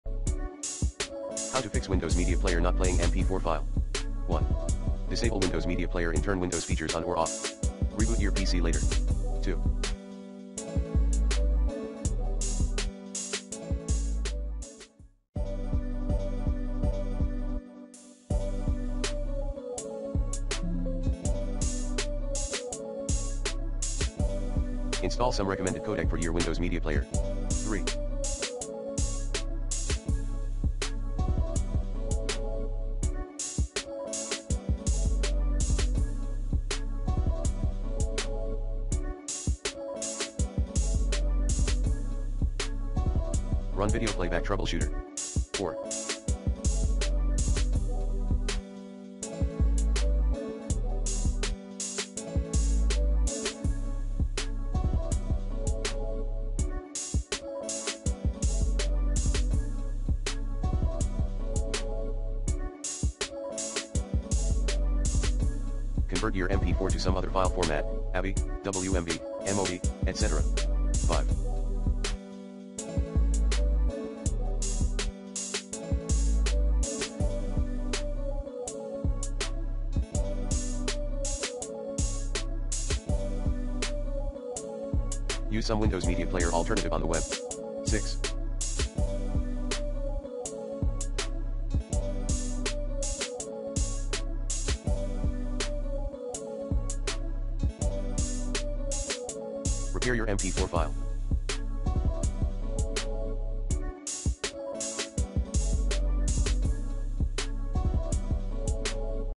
Fix Windows Media Player not playing Mp4 file on Windows
Here's how to Fix Windows Media Player not playing Mp4 file on Windows.

If you're having trouble playing MP4 files in Windows Media Player, here are some possible solutions:

1. Install missing codecs: MP4 files require specific codecs to be installed on your computer in order to play properly in Windows Media Player. You can try installing a codec pack, such as the K-Lite Codec Pack or the Combined Community Codec Pack, which include a wide range of codecs for different file types.

2. Update Windows Media Player: Make sure you have the latest version of Windows Media Player installed, as newer versions may have better support for playing MP4 files.

3. Use a different media player: If you're still having trouble playing MP4 files in Windows Media Player, you can try using a different media player, such as VLC Media Player or Media Player Classic, which are known for their wide range of file format support.

4. Convert the MP4 file to a different format: If all else fails, you can try converting the MP4 file to a different format that is supported by Windows Media Player, such as WMV or AVI. There are many free and paid video conversion software available online, such as HandBrake and Freemake Video Converter.

By trying these solutions, you may be able to fix the issue of Windows Media Player not playing MP4 files on your Windows computer.